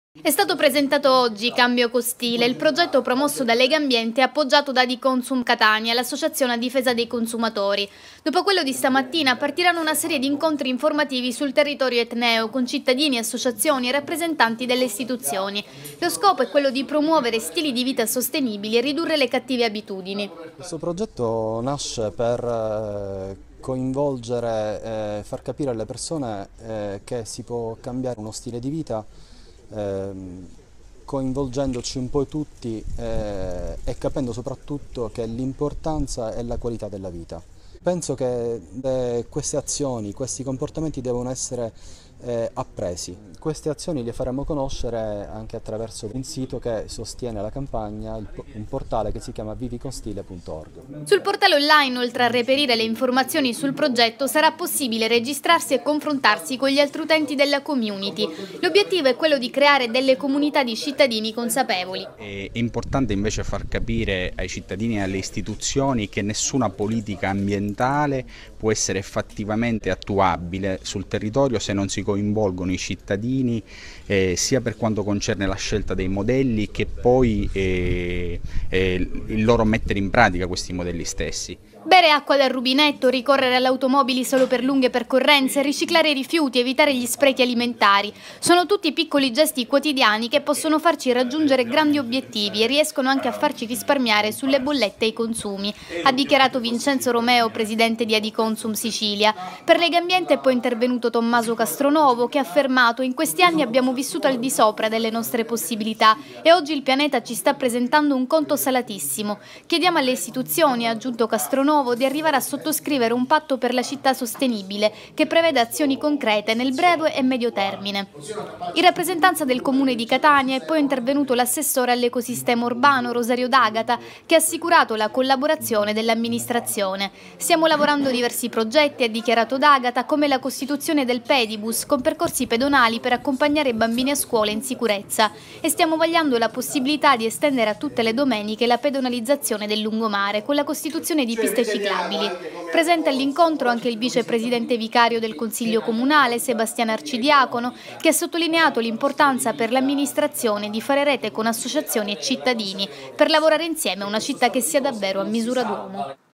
Sostenibilità Ambientale: parte "Cambio con stile", la campagna di sensibilizzazione dei cittadini.
Il progetto promosso da Legambiente e appoggiato da Adiconsum, è stato presentato oggi nella sede del centro servizio volontariato etneo in via Teseo 14 a Catania.